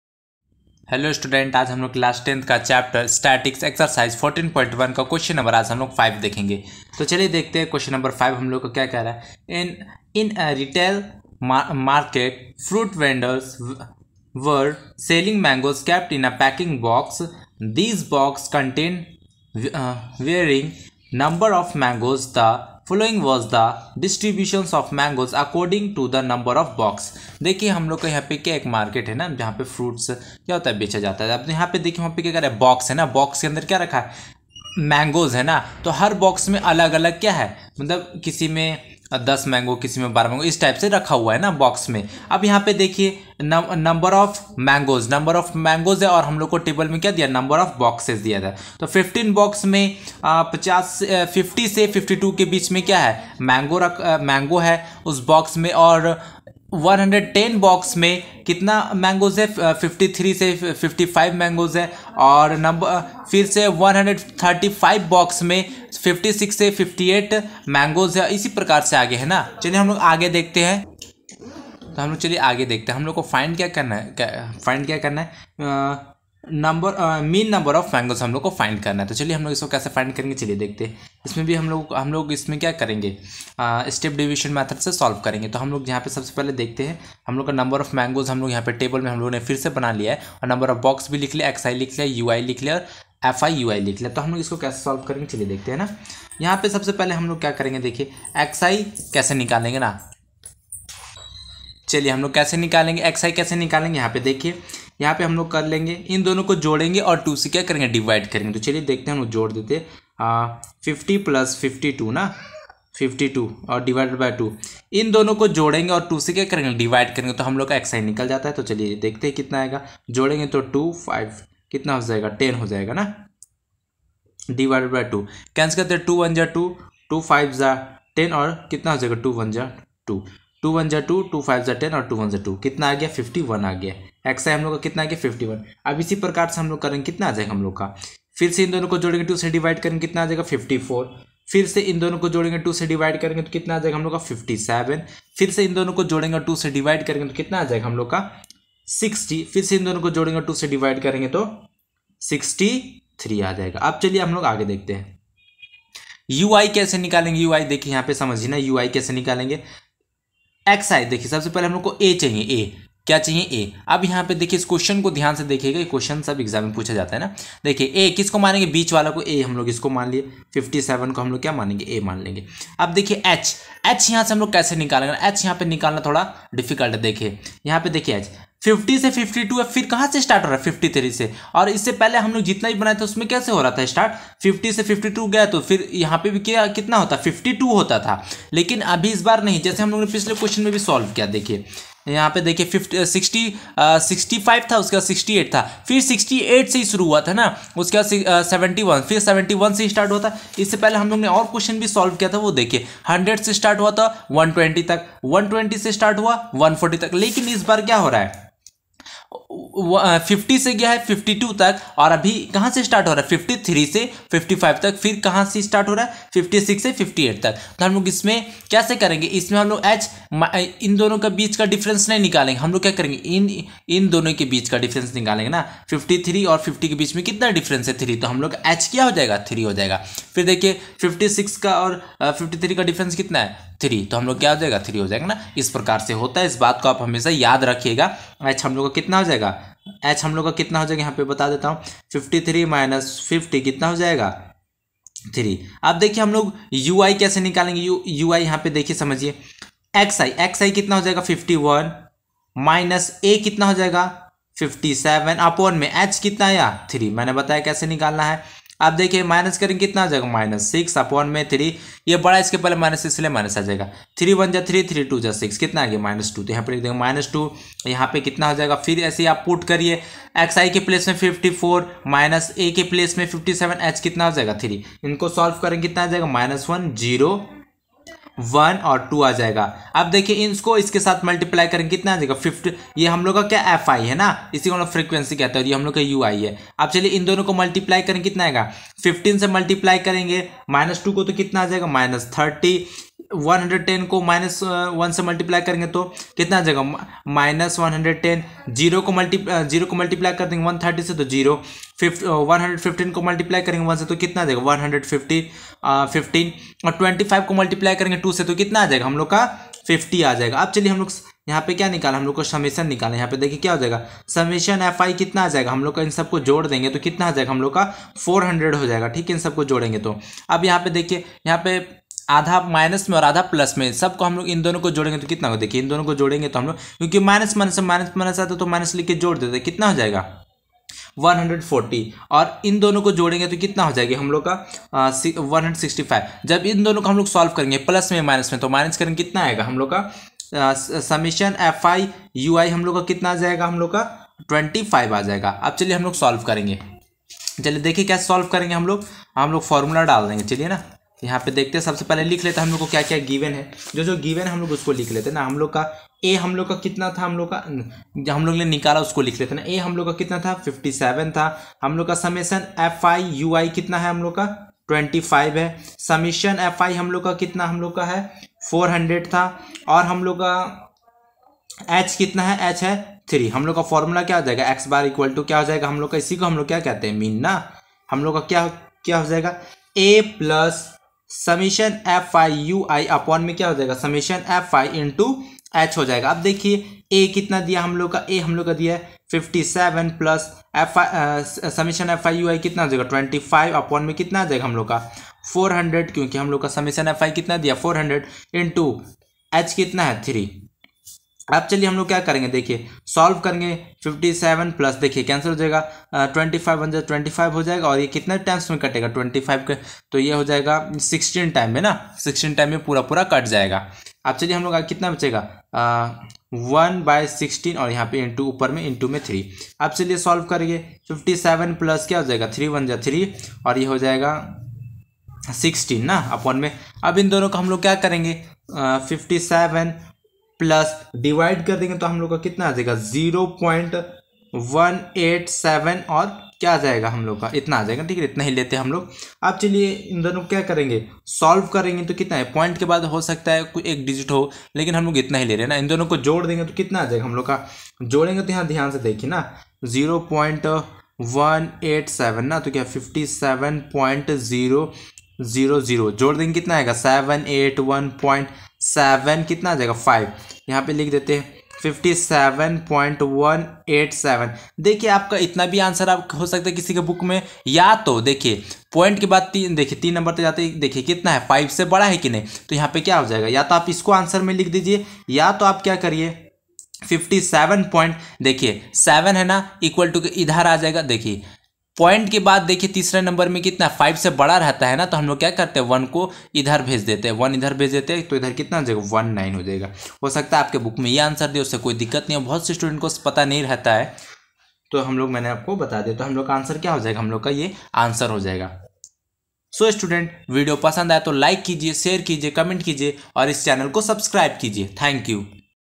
In a retail market, fruit vendors were selling mangoes kept in packing boxes. There boxes contained
Class 10 || Chapter Statistics Ex :- 14.1 || Question no.5 In a retail market, fruit vendors were selling mangoes kept in packing boxes. There boxes contained varying number of mangoes. The following was the distribution of mangoes according to the number of boxes.